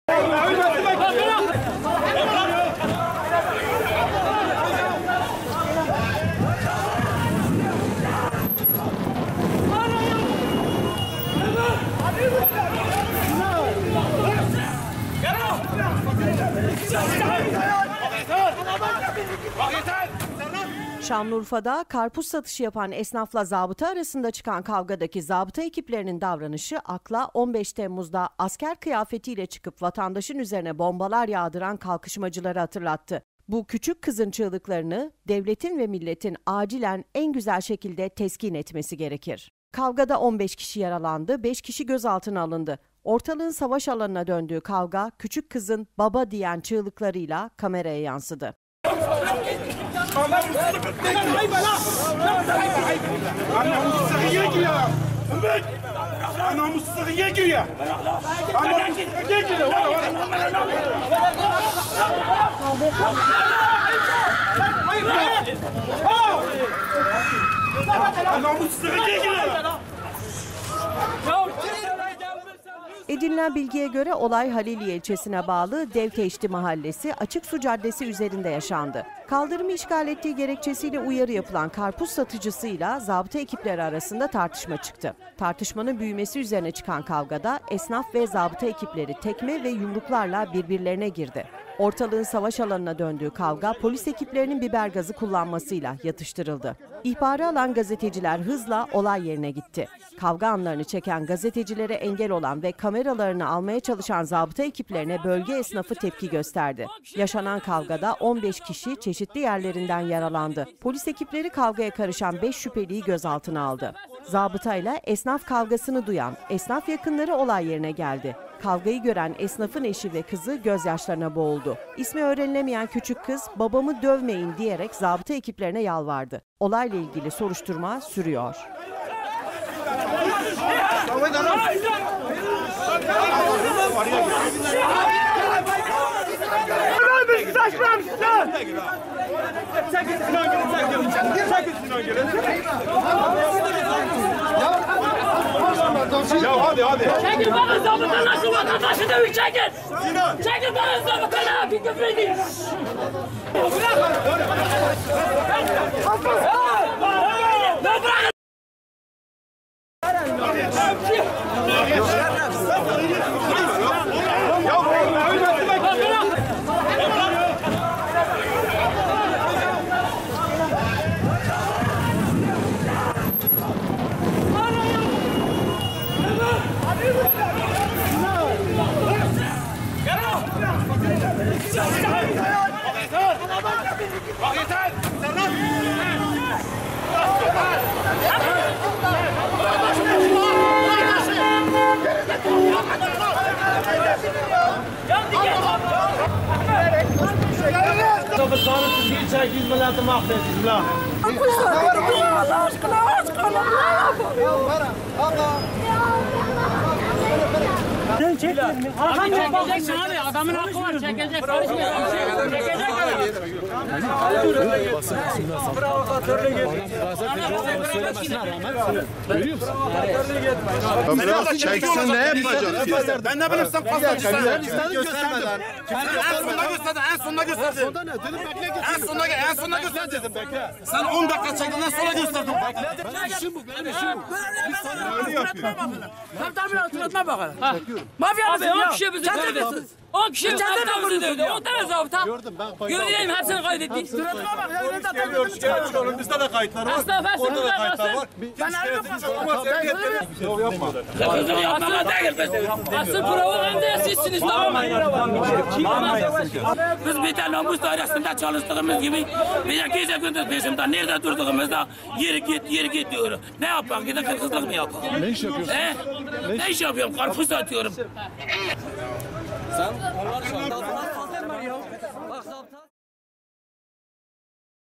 Küçük Kız Babamı Dövmeyin Diyerek Zabıta Ekiplerine Yalvardı
Şanlıurfa'da karpuz satışı yapan esnaf ile zabıta arasında çıkan kavgadaki zabıta ekiplerinin davranışı akla 15 temmuz'da asker kıyafetiyle çıkıp vatandaşın üzerine bombalar yağdıran kalkışmacıları hatırlattı, bu küçük kızın çığlıklarını devletin ve milletin acilen en güzel şekilde teskin etmesi gerekir.

kavgada 15 kişi yaralandı, 5 kişi gözaltına alındı. Ortalığın savaş alanına döndüğü kavga kuçuk kızın baba diyen çığlıklarıyla kameraya yansıdı…..

Edinilen bilgiye göre olay, Haliliye ilçesine bağlı Devteyşti Mahallesi Açıksu Caddesi üzerinde yaşandı. Kaldırımı işgal ettiği gerekçesiyle uyarı yapılan karpuz satıcısı ile zabıta ekipleri arasında tartışma çıktı.

Tartışmanın büyümesi üzerine çıkan kavgada esnaf ve zabıta ekipleri tekme ve yumruklarla birbirlerine girdi. Ortalığın savaş alanına döndüğü kavga polis ekiplerinin biber gazı kullanması ile yatıştırıldı.

İhbarı alan gazeteciler, hızla olay yerine gitti. Kavga anlarını çeken gazetecilere engel olan ve kameralarına almaya çalışan zabıta ekiplerine, bölge esnafı tepki gösterdi.

Yaşanan kavgada 15 kişi çeşitli yerlerinden yaralandı. Polis ekipleri, kavgaya karışan 5 şüpheliyi gözaltına aldı.

Zabıta ile esnaf kavgasını duyan esnaf yakınları olay yerine geldi. Kavgayı gören esnafın eşi ve kızı gözyaşlarına boğuldu. İsmi öğrenilemeyen küçük kız, "babamı dövmeyin" diyerek zabıta ekiplerine yalvardı. Olayla ilgili soruşturma sürüyor.